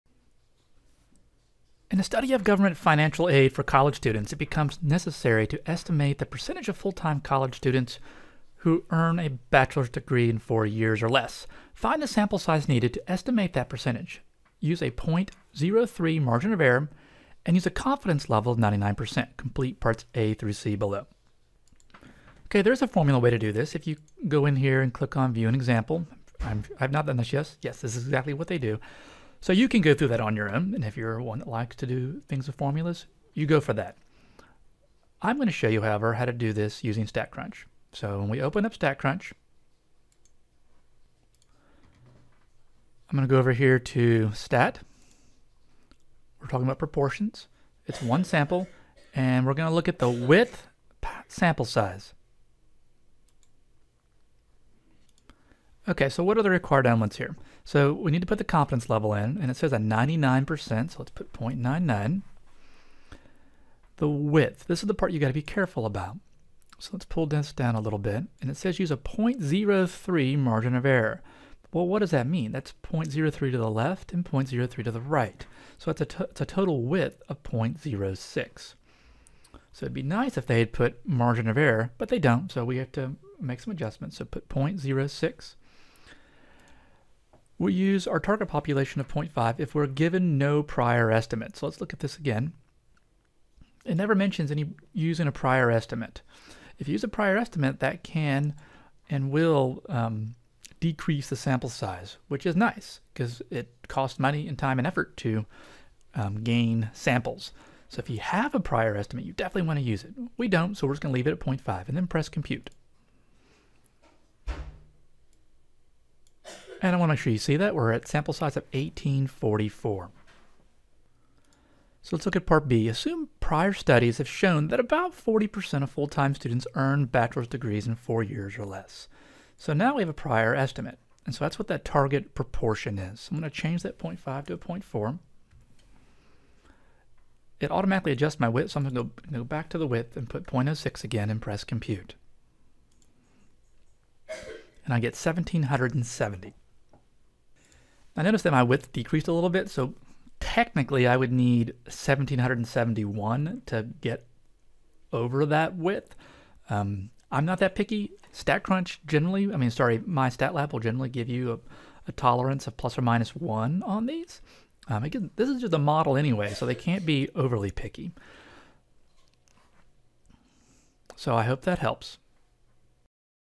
7.1.33T Sample Size needed
How to use StatCrunch to find the sample size needed for a one sample proportion for Triolas text in MyStatLab/MyMathLab